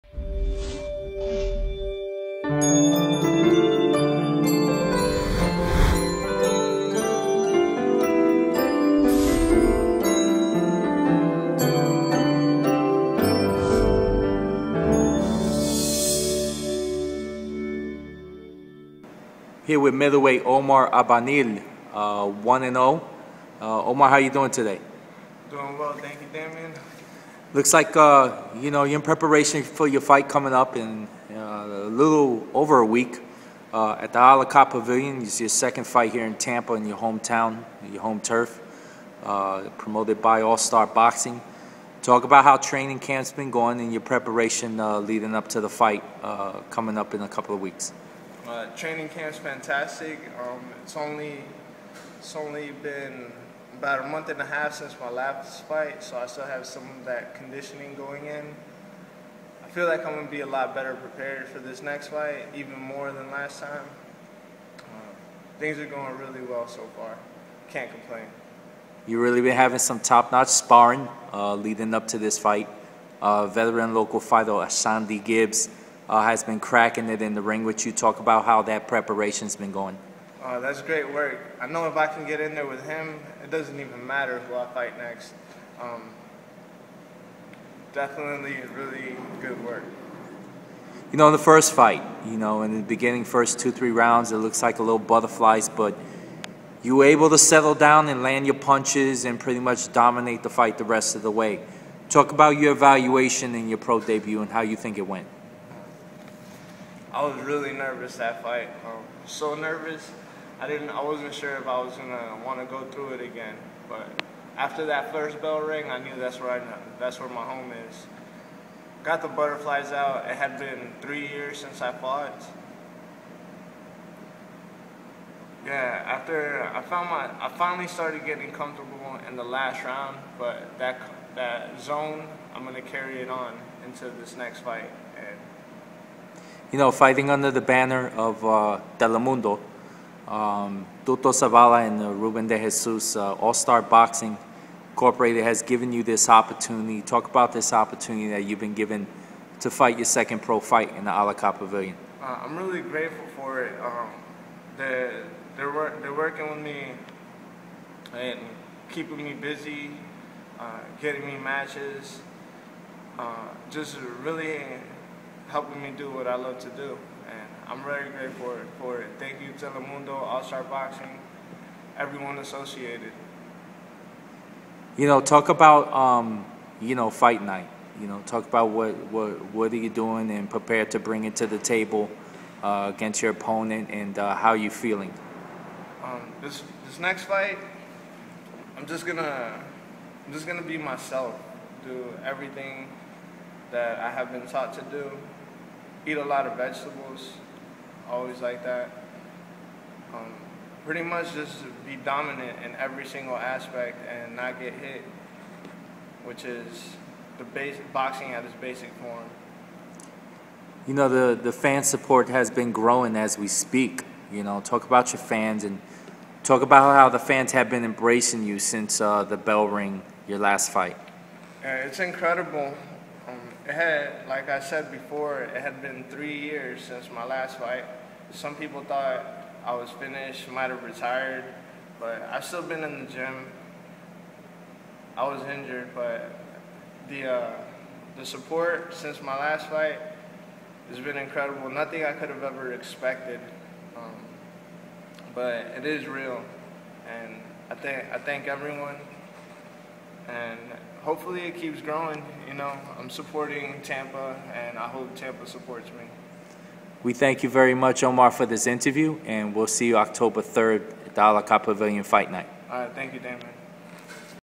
Omar Albanil Interview with Latinbox
Middleweight Omar Albanil (1-0) interview with Damon Gonzalez from Latinbox Sports. As Albanil prepares to step into the ring for his second professional fight on Friday October 3, 2014 at the A La Carte Pavilion in Tampa, Florida, presented by All Star Boxing,Inc. & Pier 6 Promotions - Boxeo Telemundo.